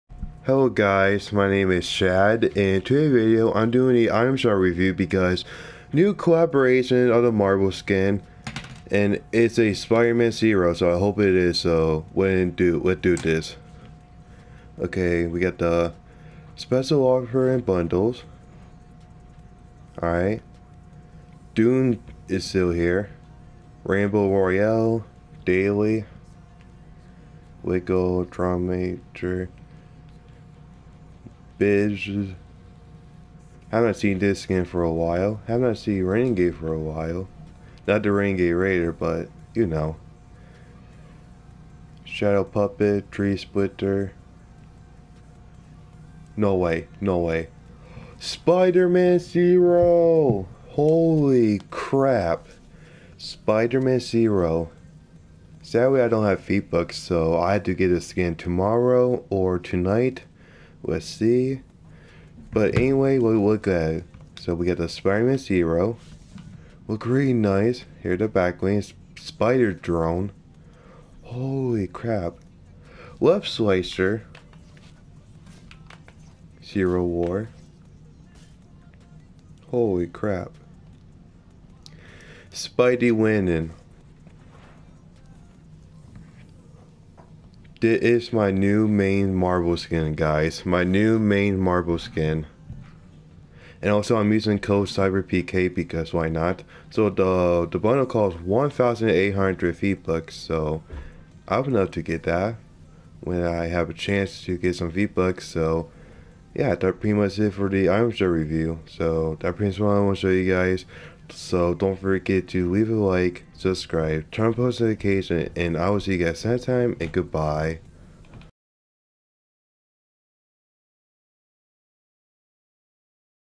SPIDER MAN ZERO (My New Main Marvel Skin)
Spider-Man Zero is FINALLY HERE! enjoy the Item Shop Review or Item Shop Showcase